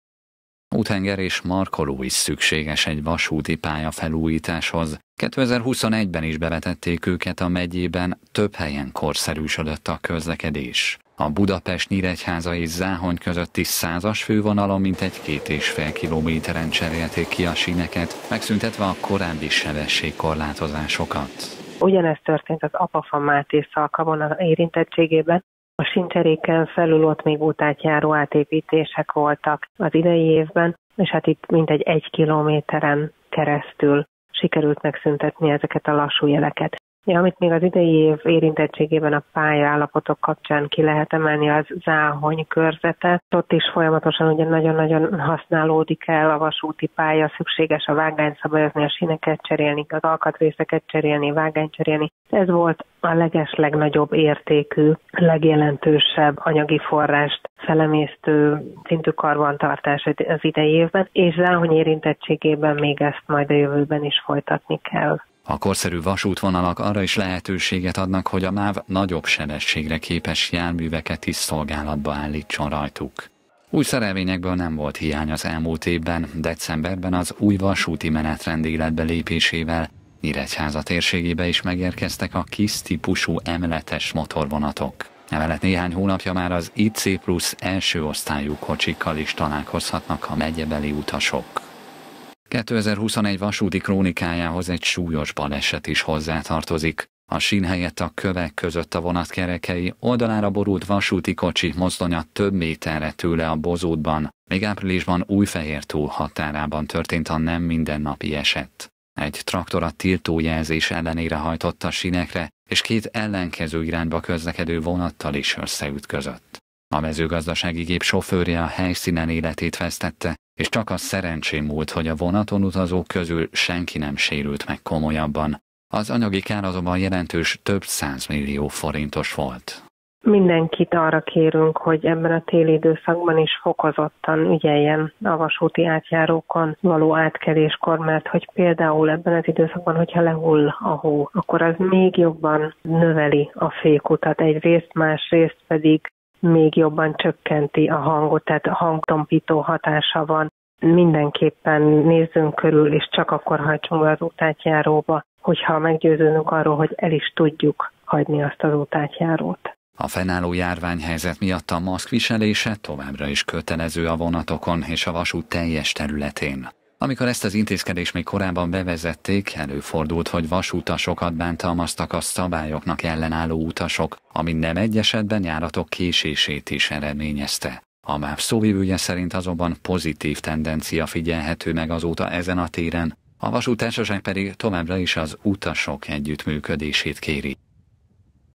Megújult vasútvonalak, új szerelvények
Síneket cseréltek, vágányokat szabályoztak - több vasútvonal is korszerűsödött 2021-ben, a megyében. Az érintett helyeken így gyorsabban közlekedhetnek a szerelvények, csökkentve ezáltal a menetidőt. A térségben utazók emellett új vasúti kocsikkal is megismerkedhettek az elmúlt évben.